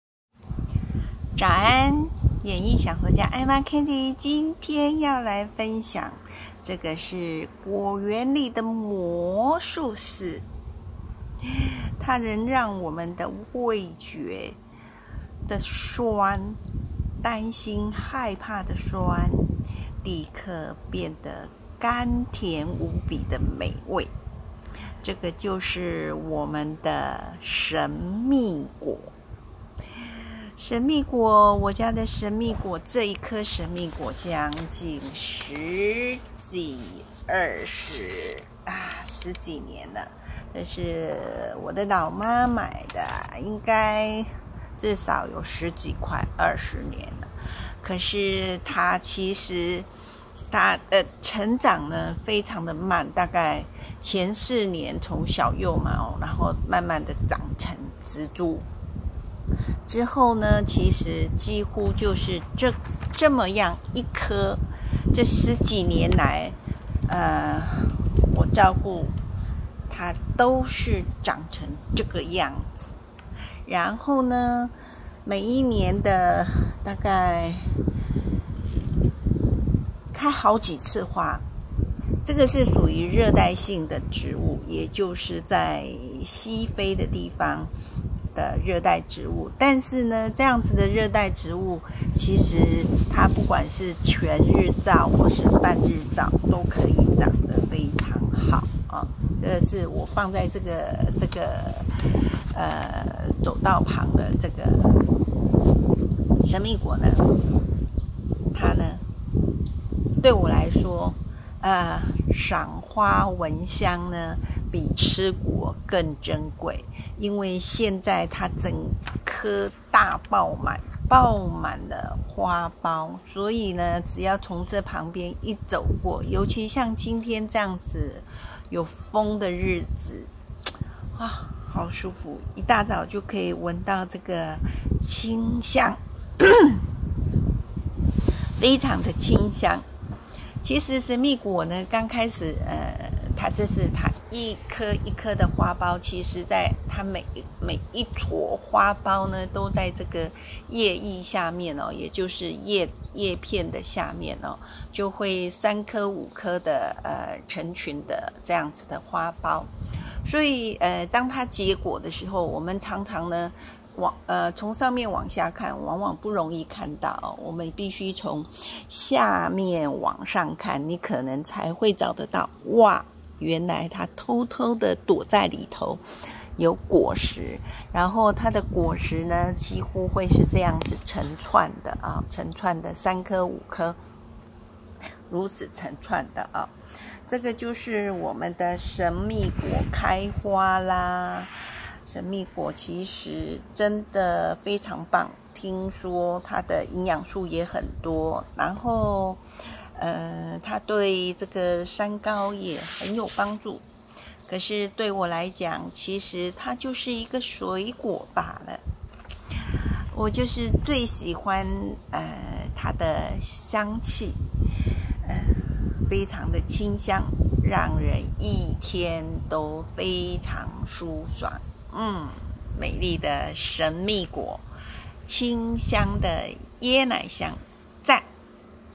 【神秘果】讓你大口吃檸檬都變香甜可口
果園裡的魔術師-神秘果花開葉腋下，全年開花，花有淡淡椰奶香， 成熟的果粒深紅色的小果，裡面有一種種子少量白色甜味的果肉，深紅果實有如釋迦果的種子， 吃一顆神秘果就可把檸檬酸變為甘甜無比讓味覺得口腔甜滋滋的。